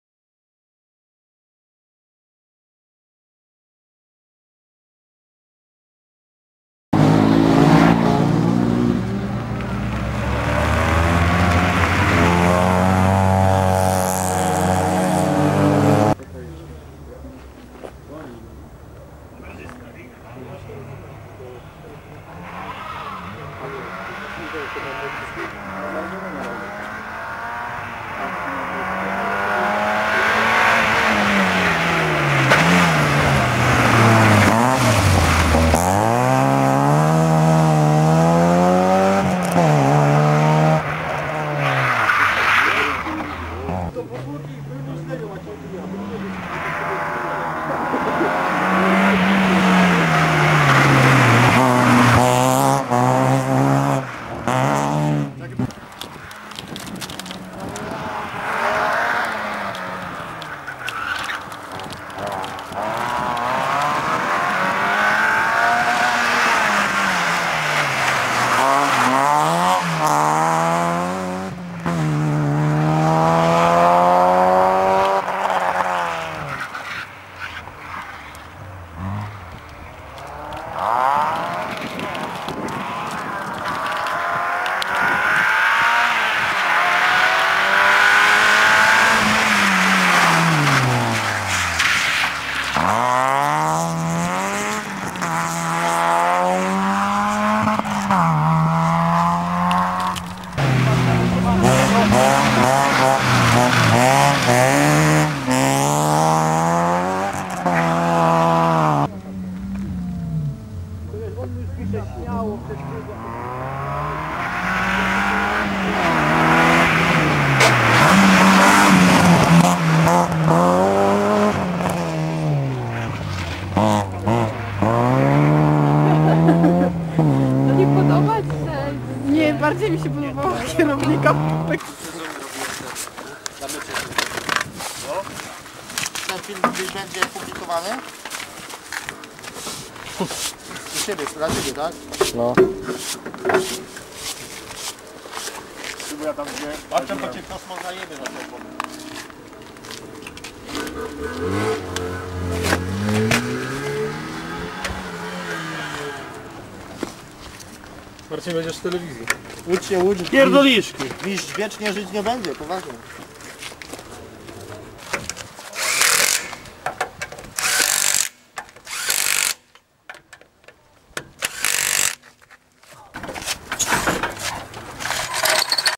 VII Super Sprint Chorzow 19.10.08 Szczypuła/Gańko
przejazd zalogi, ktora w najblizszym roku ma zamiar toczyc walke w PPZM... Pozostaje nam tylko ich goraca dopingowac i zyczyc szerokiej drogi